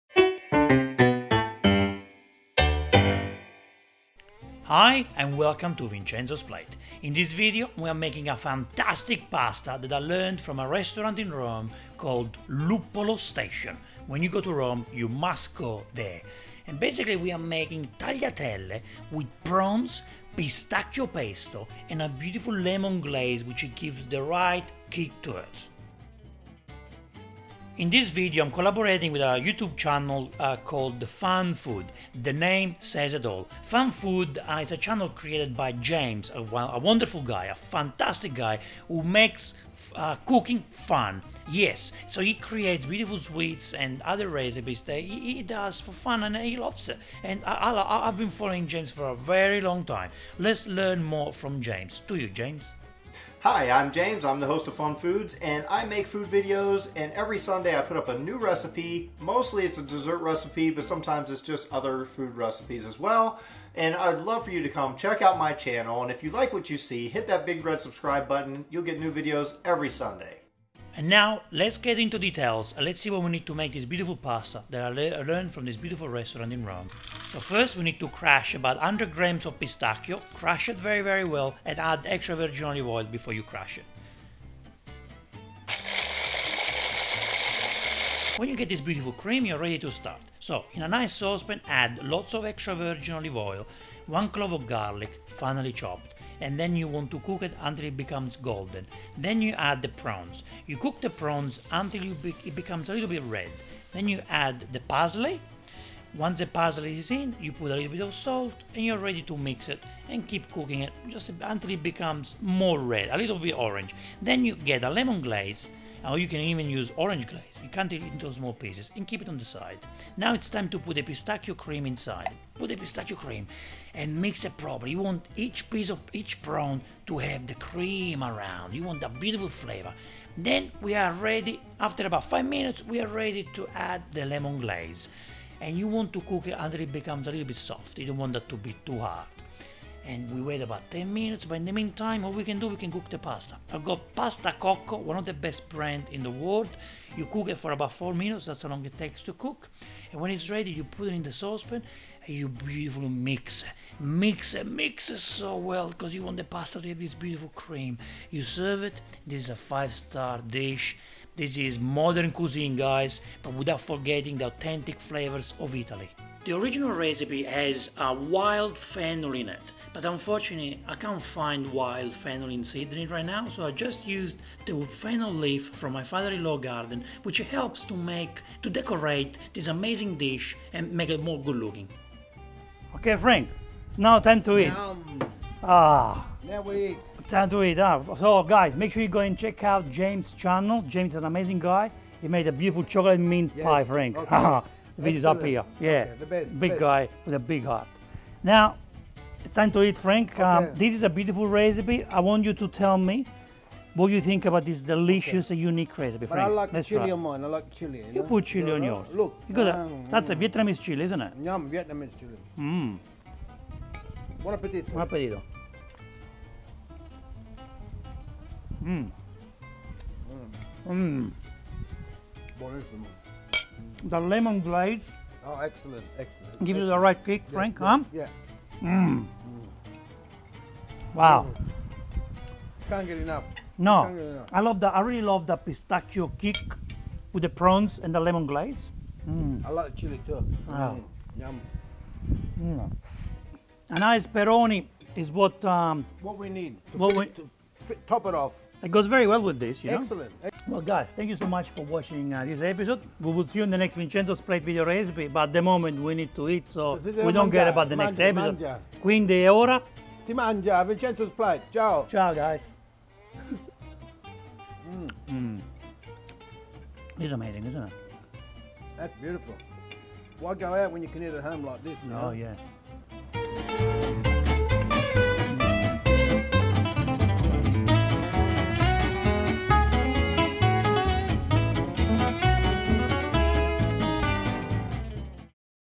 PRAWN PASTA | How To Make PRAWNS (Shrimp) & PISTACCHIO Pasta | Seafood Pasta Recipe
Prawn Pasta Recipe.Heavenly strands of tagliatelle pasta covered in fresh pistacchio pesto, mixed with prawns (shrimps) and finished with a lemony glazed zest. Get your mouth around this combination of nutty textures and seafood.
Seafood Pasta Recipe, Seafood Fettuccine. ITALIAN SEAFOOD PASTA